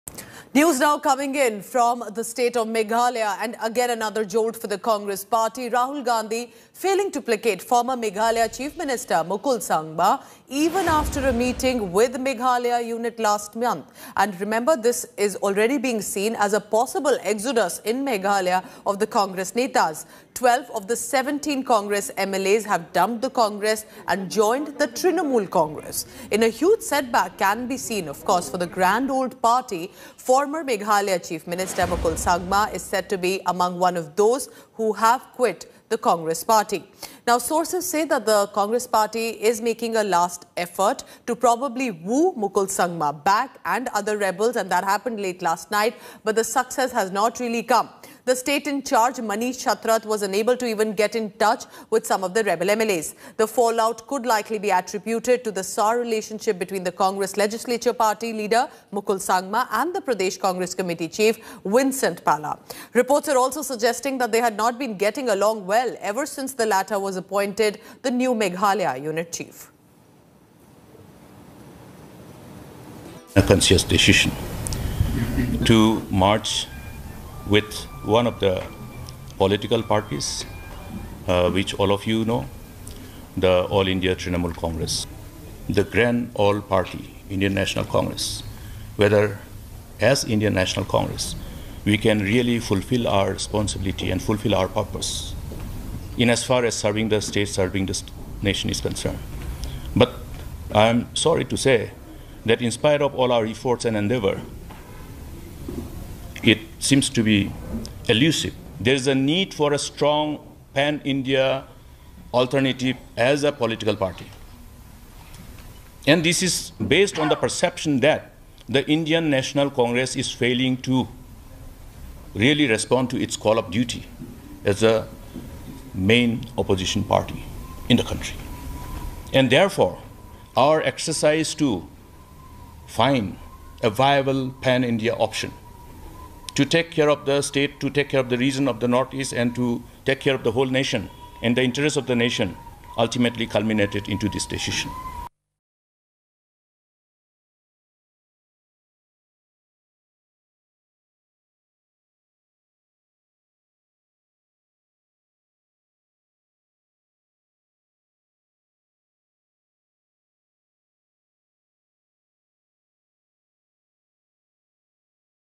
Jolt To Congress As Mukul Sangma, 11 Meghalaya MLAs Join TMC
In a massive jolt to Congress, former Meghalaya Chief Minister Mukul Sangma and 11 others joined Trinamool Congress in Meghalaya. With this move, 12 out of 17 Congress MLAs joined Trinamool. This comes a month after Rahul Gandhi met former Meghalaya Chief Minister Mukul Sangma and the party's state president Vincent H Pala over their demands. Watch the video.